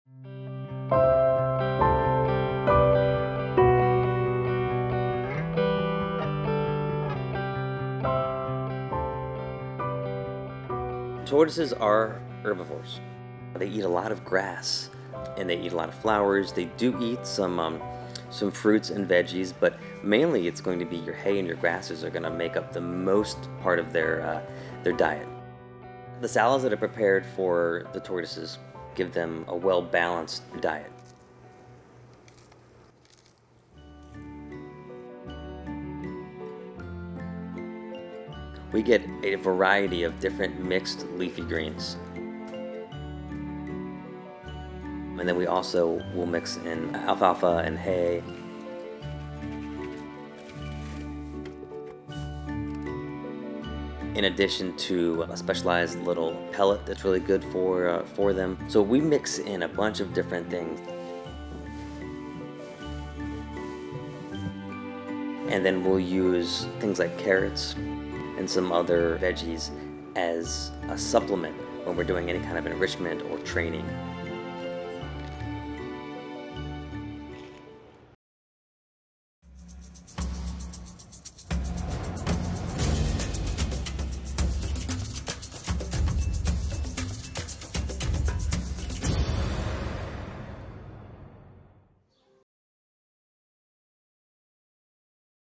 What Do Tortoises Eat?
Consummate herbivores, tortoises have a healthy and hearty diet at Smithsonian's National Zoo. Here, caretakers walk us through what goes into their special salad mix.